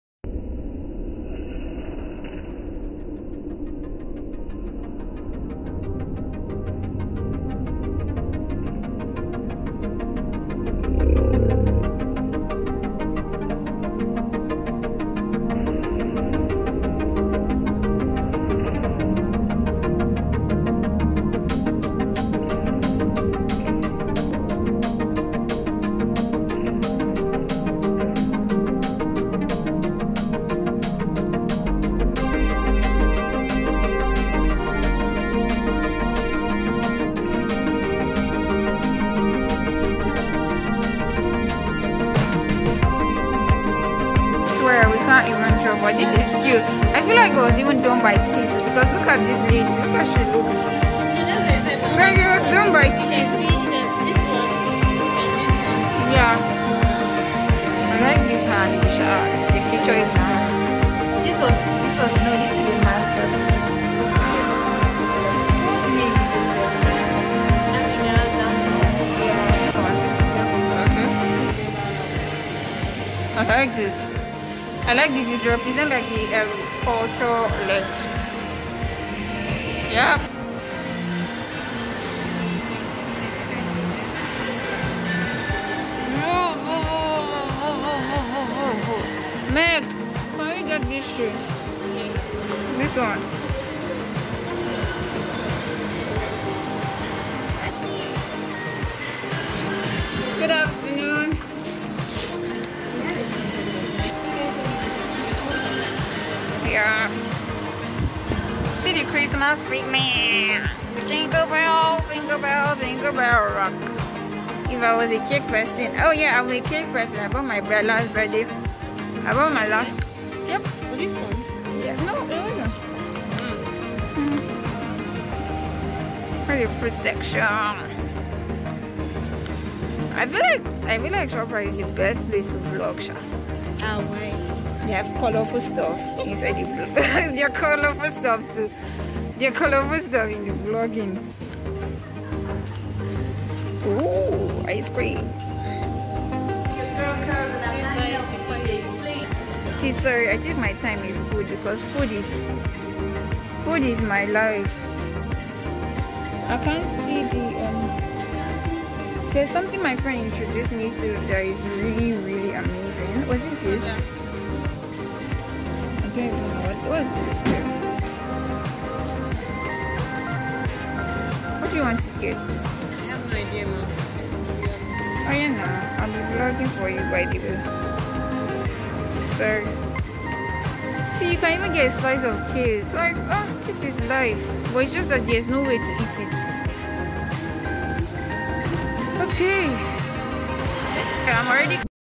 ART ||GALLERY|| SHOPRITE|| FOOD!!!!
Went to Shoprite Art Gallery in wuse zone 5 Abuja to see the beautiful paintings done by the school kids which hold so much Hope for Nigeria.
So Nigerians chin up there is HOPE!.